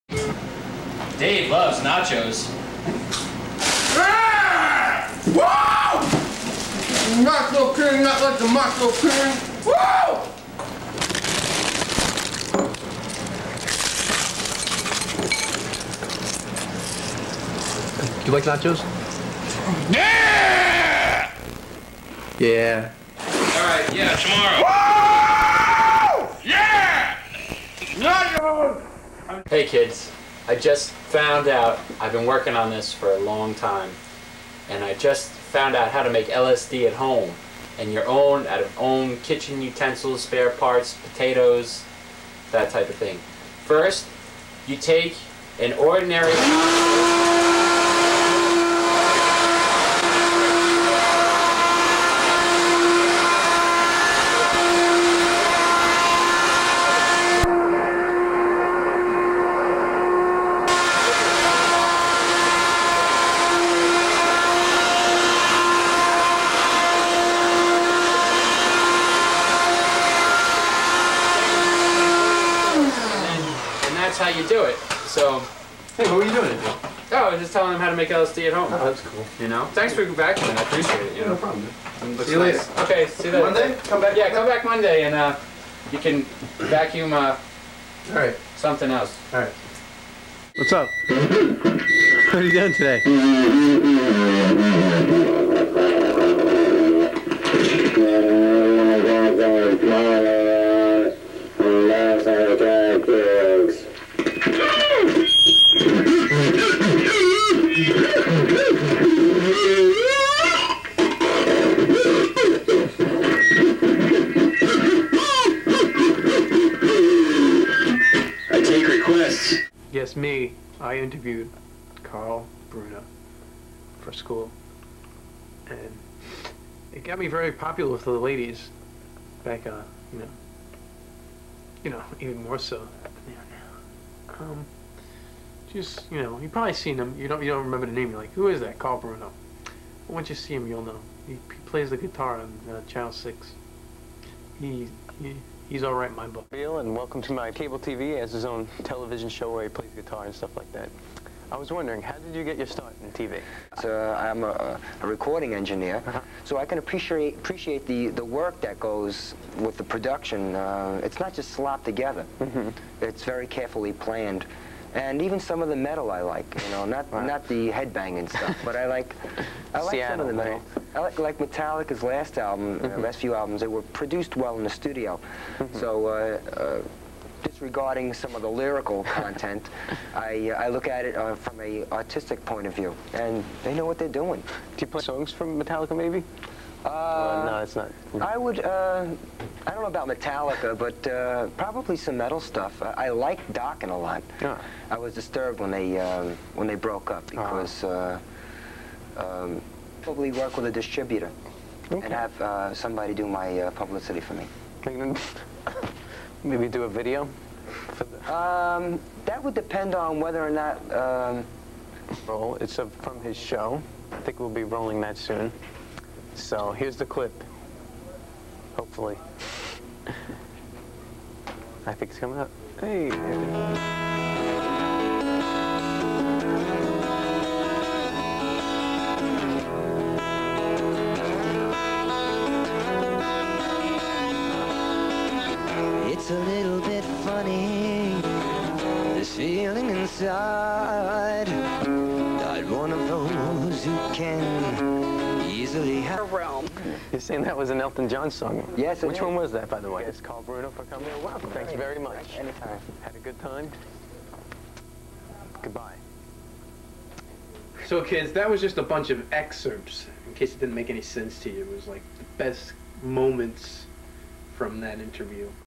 Broasted Chicken-Dave Loves Nacho's/Make LSD at Home/The Throatalist/Greg interviews Carl Bruno
Dave is the Nacho King not like the Macho King. We share with you how to make LSD at home using household materials,  I make some dumb throat noises and Greg interviews Long Island/Brookhaven Cable Legend Carl Bruno.
Production Note: Carl Bruno is a great sport and this is no way to make fun of him. He was nice enough to allow Greg to interview him for a Boces project. I think he is finally over Dokken breaking up though.